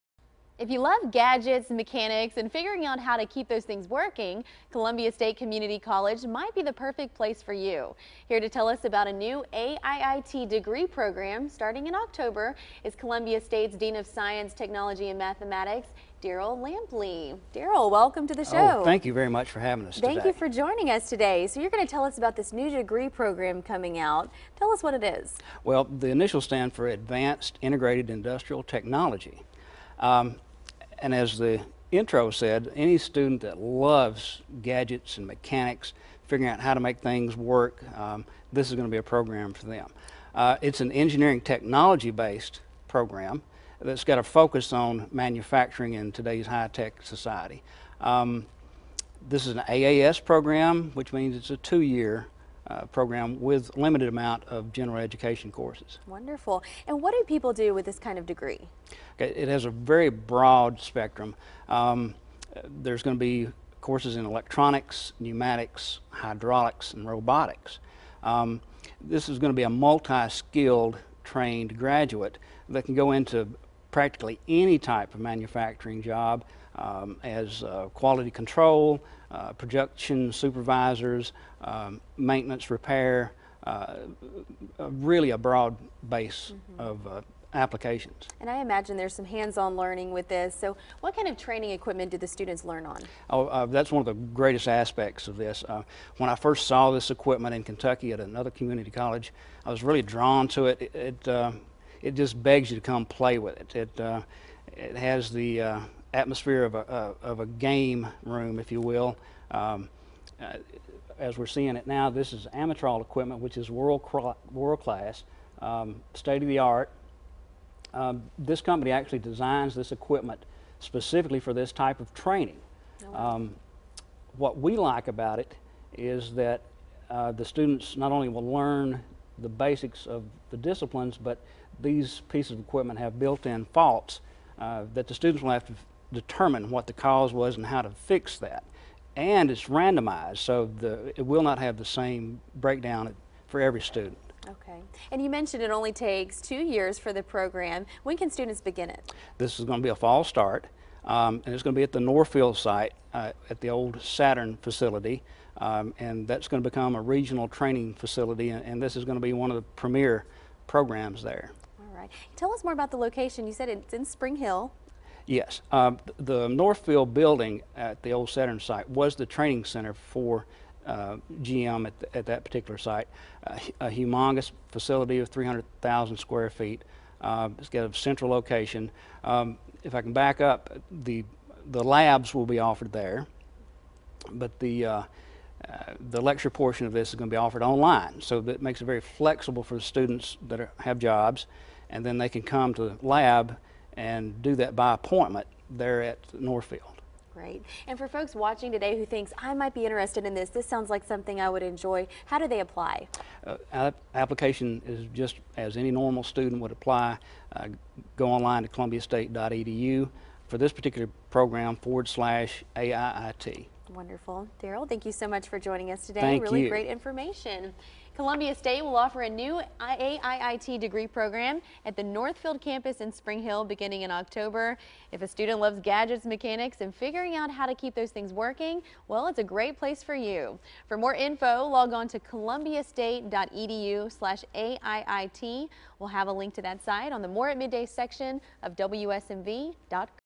Columbia State Community College on WSMV Channel 4 News - AiiT Program
Dearl Lampley, Dean of the Science, Technology and Math Division at Columbia State Community College recently appeared on WSMV's More at Midday Show to talk about the new Advanced Integrated Industrial Technology (AiiT) program. For more information about the AiiT program